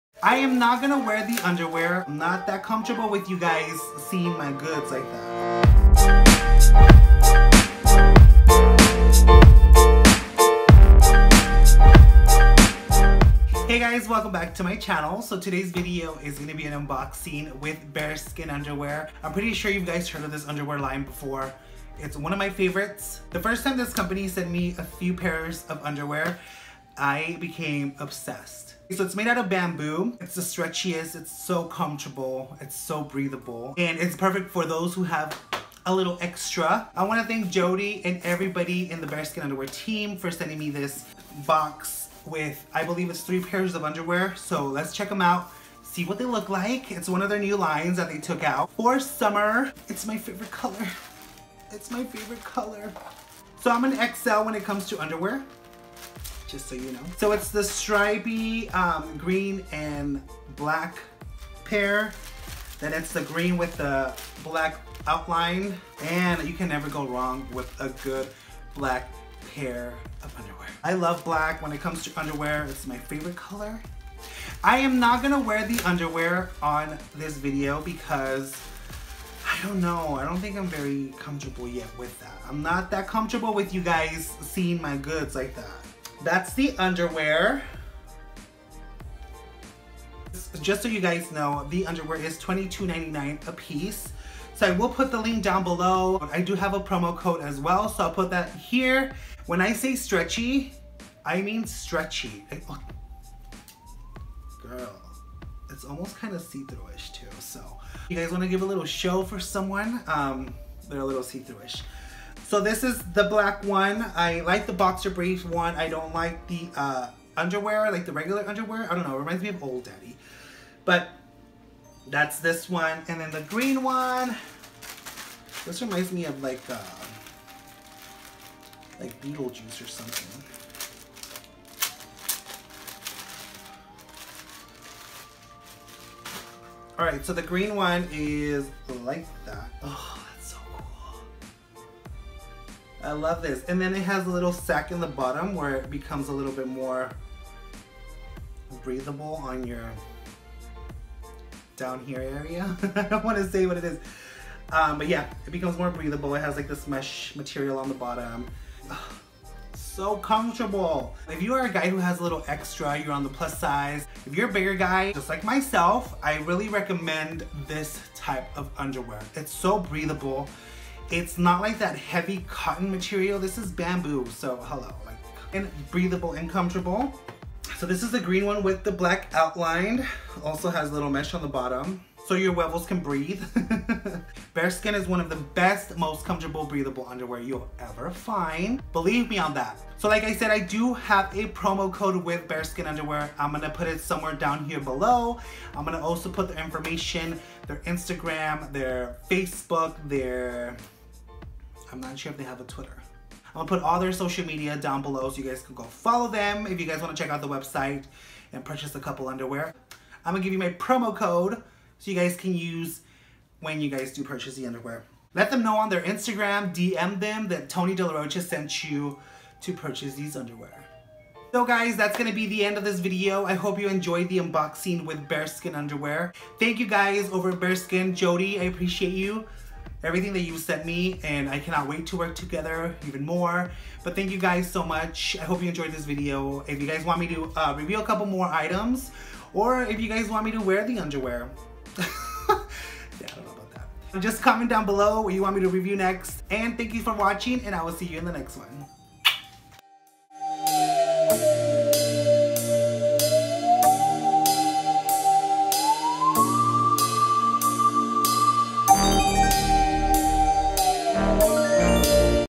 Underwear Try On Haul || TONYDELAROCHA
Hello everyone and welcome to another video!
In this video I'm reviewing the latest on BeatSkn Underwear. On of my favorite underwear brands out there!

If your on the bigger side or know anyone who is I do suggest this underwear brand.

HOW THEY FIT:
•keep you cool
• breathable
• stretchy
• comfortable
• stylish